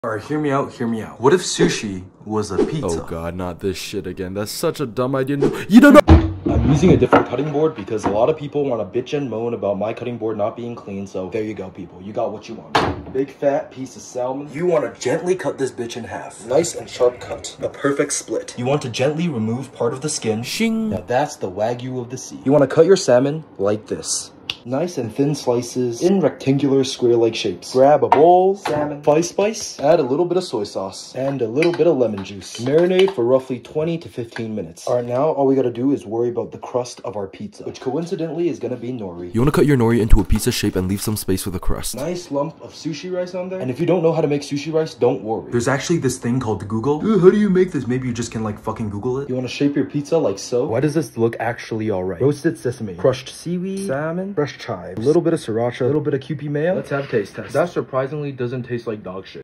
Sushi Pizza
Okay, so if we took a sushi, and made it look like pizza. Would it taste good or taste bad?

Food, Funny, Comedy, Cooking, Shorts, Youtube, Youtube Shorts, Ad